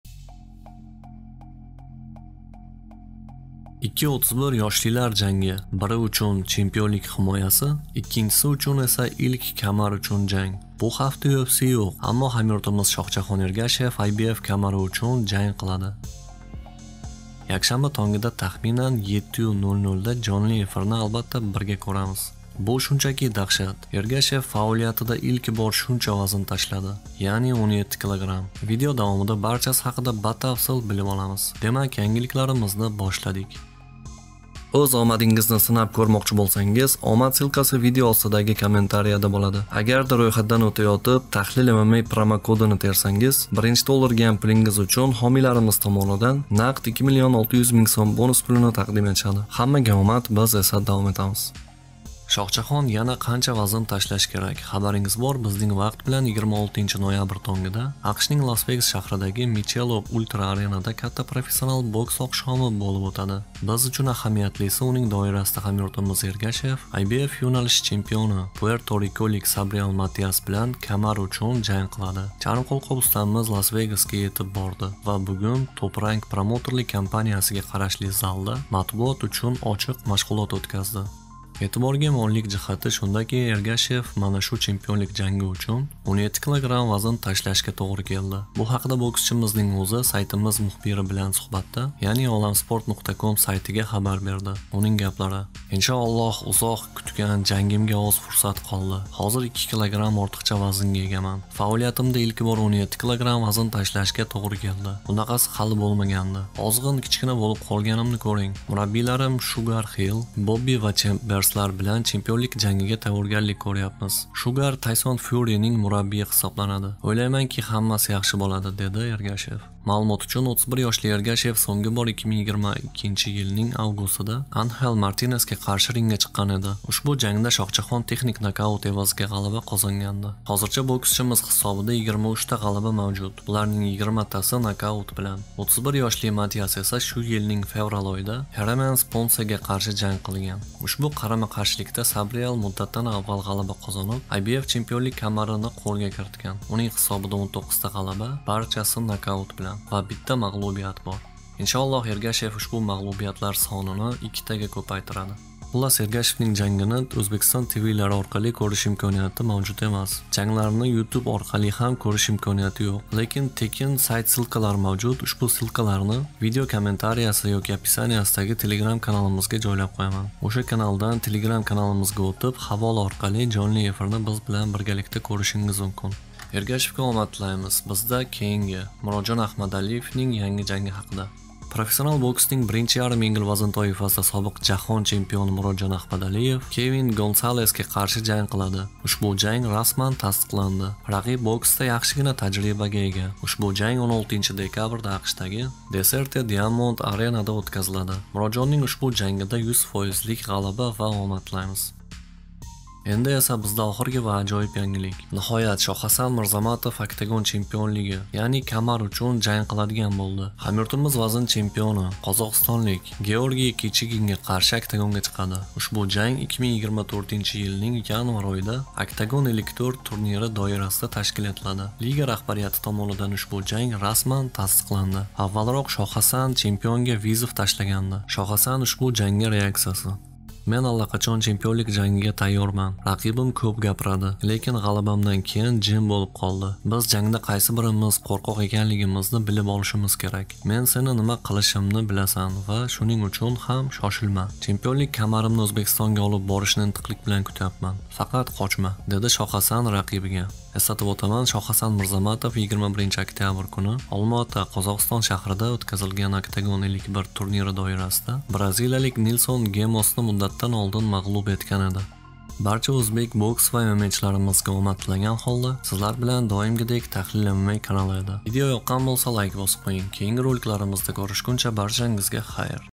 ЯКШАНБА ЯНГИ ЖАНГ! ШОХЖАХОН ЕРГАШЕВ ЖОЛИ ЭФИР!
Shu xafta xamyurtimiz Shoxjaxon ergashev chempionlik uchun jang qiladi!

Tahlil mma  sport yangiliklari birinchilardan bo'lib kuzatib boring!

*Это видео является исключительно фанатичным, если вы (владельцы) хотите удалить это видео, пожалуйста, свяжитесь с нами прямо, прежде чем что-либо делать. Мы с уважением удалим его.